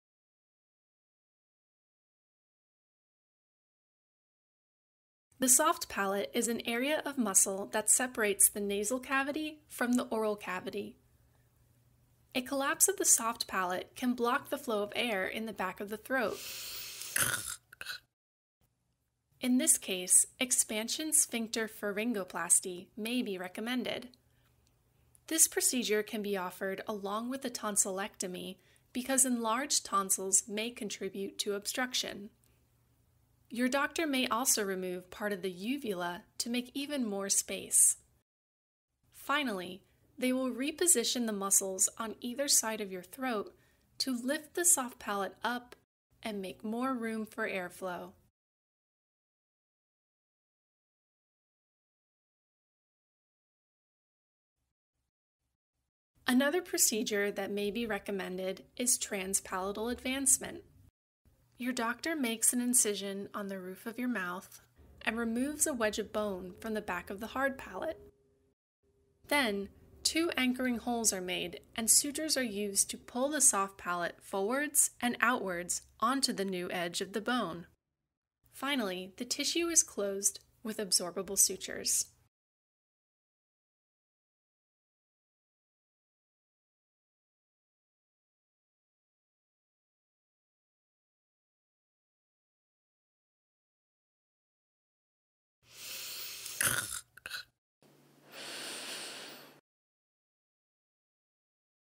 Treating Soft Palate Obstruction | Penn Sleep Surgery
This video highlights the different surgical procedures to treat soft palate collapse in patients with obstructive sleep apnea.

To learn more about the Division of Sleep Surgery at Penn Medicine, check us out through the following links: